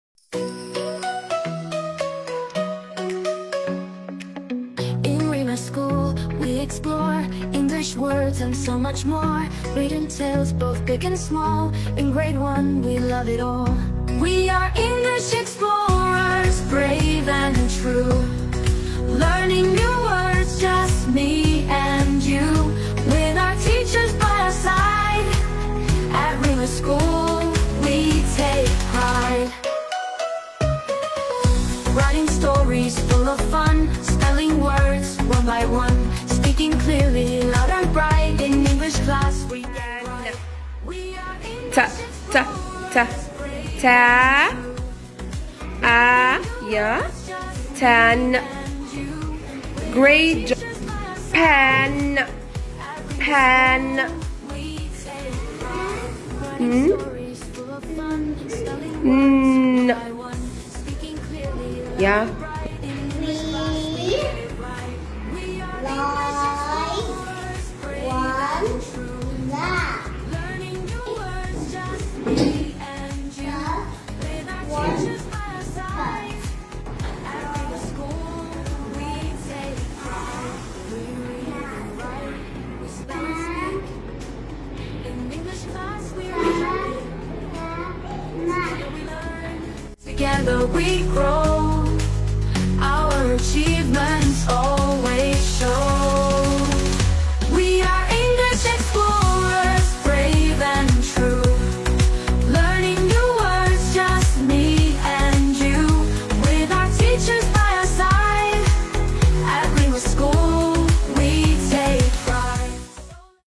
Reema_International_School Intensive Classes
📚✨ Our Grade 1 intensive students are hard at work practicing CVC words, sight words, and sentences to prepare for their reading snap story! 🌟
Grade 1i & 1E
Intensive Classes